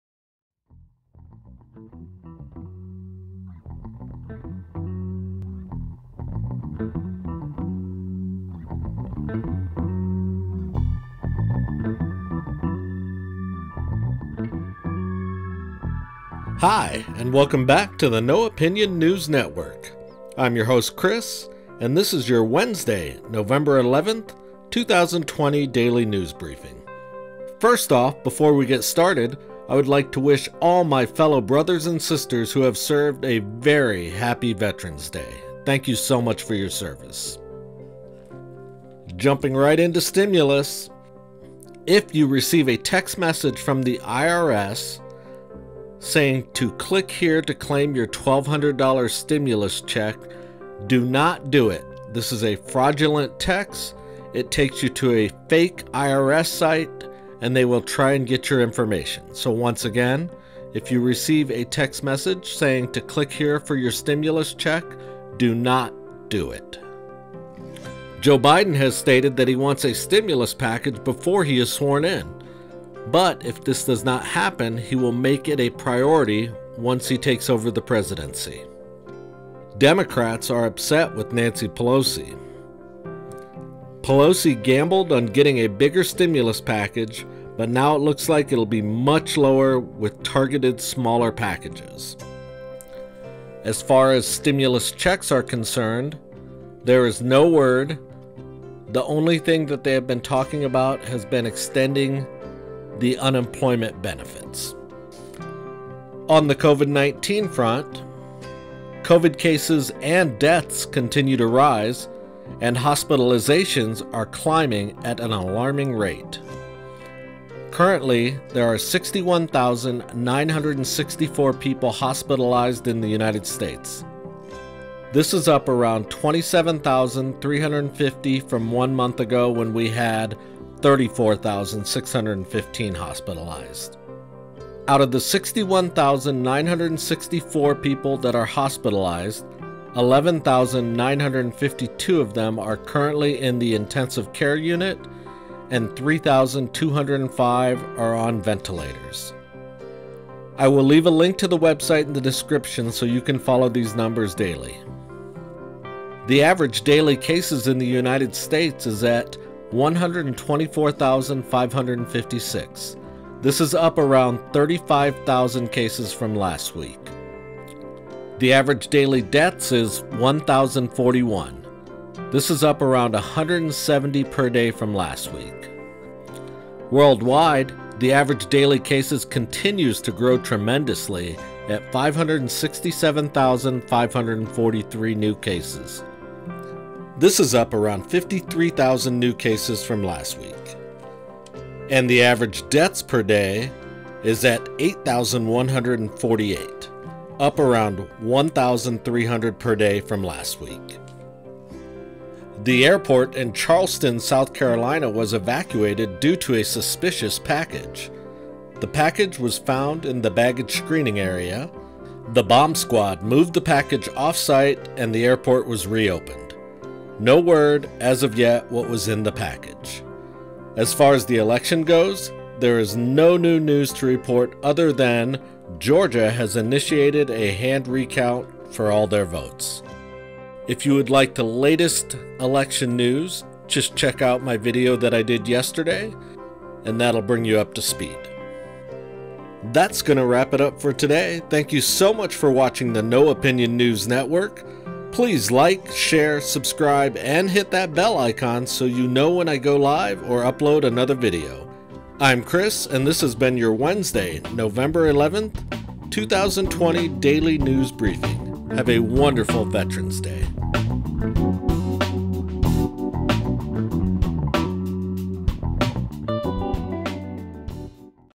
Stimulus Update, Bomb Scare at Charleston Airport, Covid-19, Election Update, Daily News 11-11
Link to Covid Hospitalization Tracking in U.S.